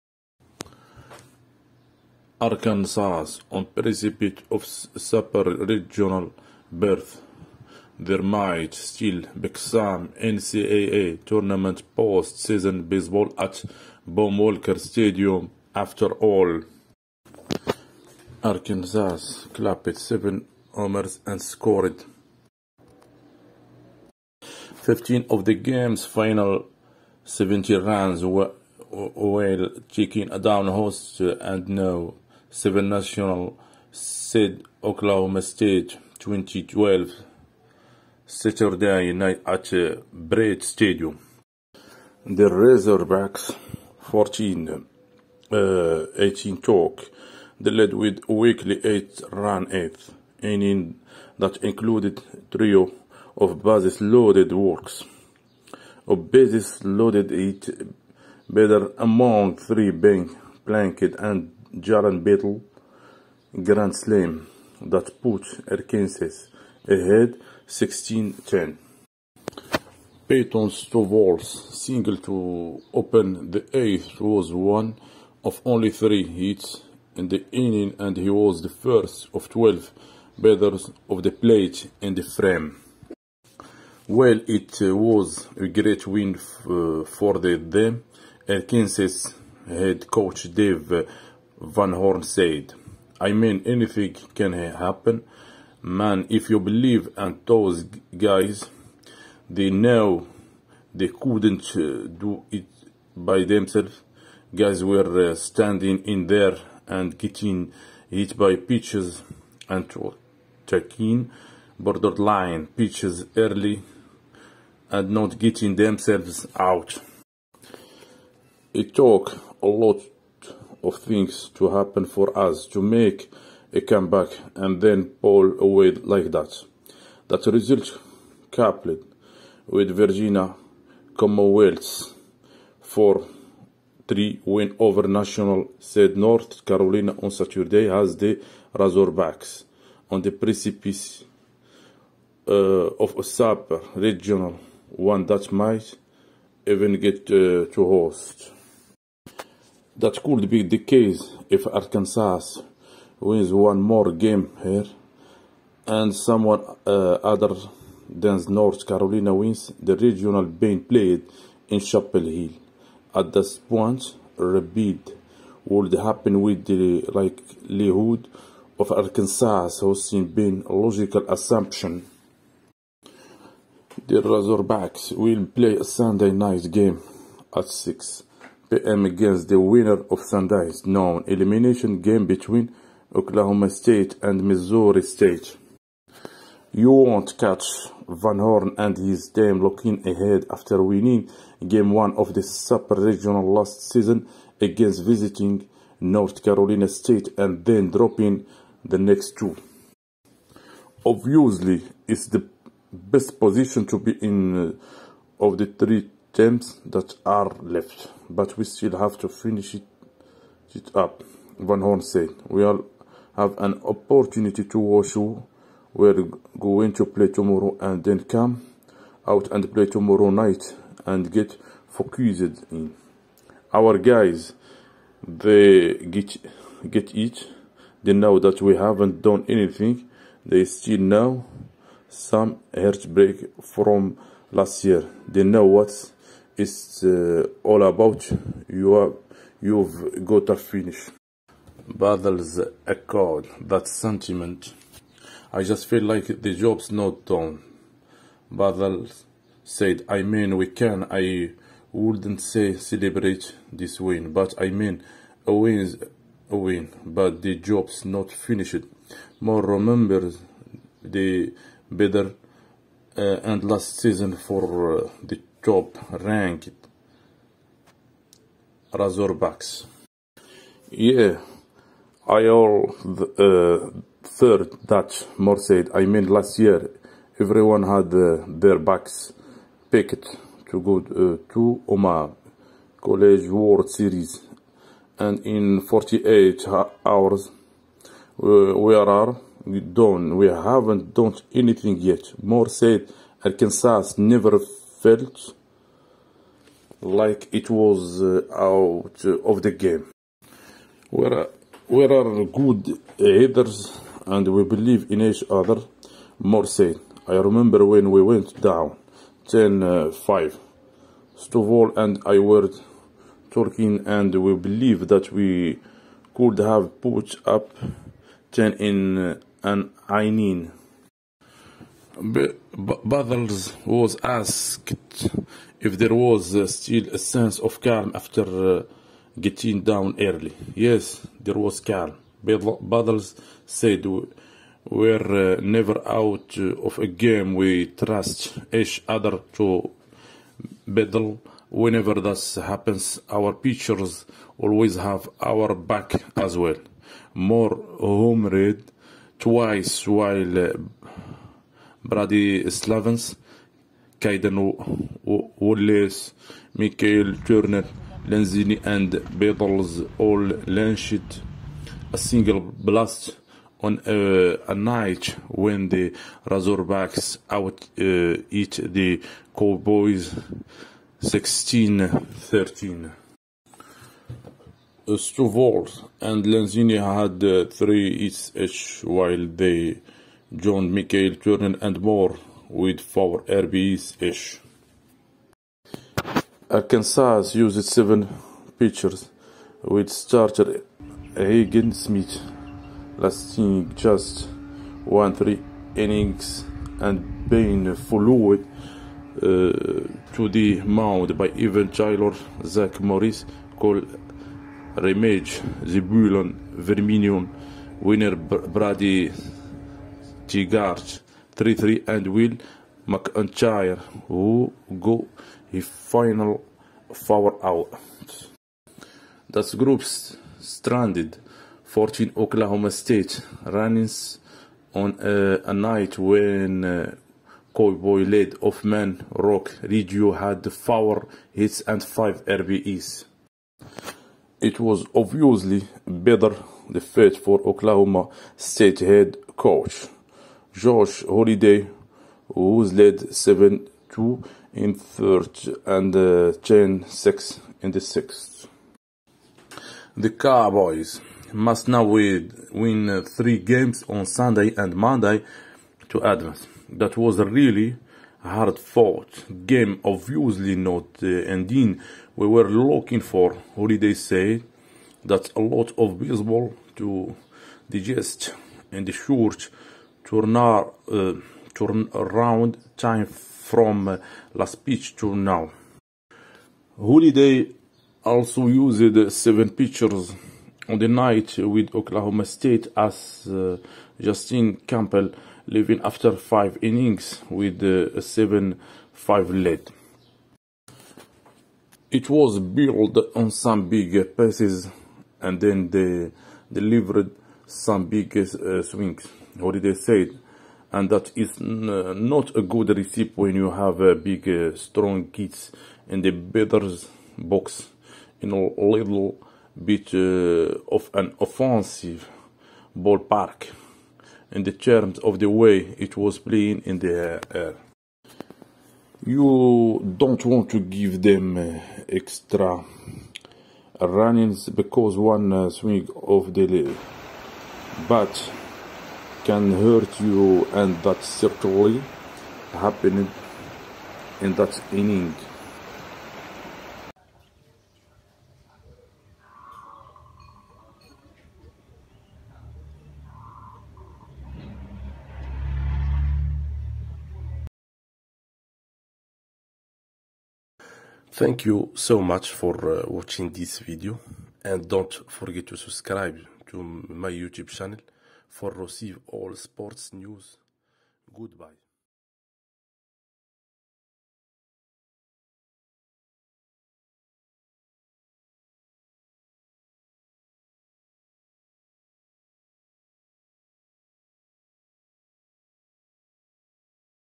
Arkansas on precipice of Super Regional be
After all, there could still be NCAA Tournament postseason baseball at Baum-Walker Stadium.

Arkansas hit seven homers and scored 15 of the game's last 17 runs while knocking out host and No. 7 seed Oklahoma State 20-12 Saturday night at O'Brate Stadium.

The Razorbacks (40-18) took the lead with a goofy eight-run eighth inning that included a trio of base-laden walks, one base-laden hitter of three dropped and Jalen Battles' grand slam that put the Arkansas ahead 16-10.

Peyton Stovall's single to open the eighth was one of only three hits in the inning and he was the first of 12 batters at home plate in the frame.

"Well, it was a great win for the team," Arkansas head coach Dave Van Horn said. “I mean, anything can happen, man, if you believe and those guys, they knew they couldn't do it on their own. Guys were standing there and getting hit by pitches and taking limit pitches early and not coming out.

"It took a lot of things to happen for us to make a comeback and walk away like this."

That result, coupled with Virginia Commonwealth's 4-3 victory over national seed North Carolina on Saturday, put the Razorbacks on the brink of a Super Regional, which could even be the host.

That could be the case if Arkansas wins one more game here and someone other than North Carolina wins the regional played at Chapel Hill.

At this point, a new offer would occur with the likelihood of hosting Arkansas being a logical assumption.

The Razorbacks will play a game Sunday night at 6 p.m. against the winner of Sunday's noon playoff game between Oklahoma State and Missouri State.

You won't see Van Horn and his team looking to the future after winning the first game of the Super Regional last season against visiting North Carolina State and then dropping the next two.

"Obviously it's the best position out of the three remaining teams, but we still have to finish it," Van Horn said. “We will have the opportunity to see who we are going to play tomorrow and then go out and play tomorrow night and concentrate.

“Our guys, they understand. They know we haven't done anything. They are still experiencing a few heartbreaks from last year. They know what it is. You must finish.

The battles echoed this sentiment.

“I just feel like the job isn't done,” Battle said. "I mean, we can, I wouldn't say celebrate this win, but I mean, a win is a win. But the work is not finished. »

Moore recalls the bitter end to last season for the top-ranked Razorbacks.

"Yeah, I'm going third," Moore said. “I mean last year everyone had packed up to go to Omaha (College World Series) and within 48 hours we were done. We haven't done anything yet."

Moore said Arkansas never felt like they were offside.

"We're good hitters and we believe in each other," Moore said. "I remember when we were down 10-5, Stovall and I were talking, and we think we could have put 10 in one set."

Battles was asked if there was still a sense of calm after going to bed early.

"Yes, there was calm," Battles said. “We are never offside. We trust each other to fight. Whenever that happens, our pitchers always have our backs too.

Moore homered twice while Brady Slavens, Cayden Wallace, Michael Turner, Lanzilli and Battles all threw a single outburst on a night when the Razorbacks beat the Cowboys 16-13.

Stovall and Lanzilli had three hits apiece as they join Michael Turner and Moore with four RBIs apiece.

Arkansas used seven pitchers with starter Hagen Smith going just 1 1/3 innings and was trailed to the mound by Evan Taylor, Zack Morris, Kole Ramage, Zebulon Vermillion, winner Brady Tygart (3- 3) and Will McEntire, who got the final four outs.

This group blocked 14 Oklahoma State runners on a night when Cowboy leading Roc Riggio had four hits and five RBIs.

It was obviously a bitter loss for Oklahoma State head coach Josh Holliday, who led 7-2 in the third and 10-6 in the sixth.

The Cowboys now need to win three games Sunday and Monday to qualify.

"It was a hard fought game, obviously not the end we were looking for," Holliday said. "That's a lot of baseball to digest in the short time between the last pitch and now."

Holliday also used seven pitchers on the night with the ace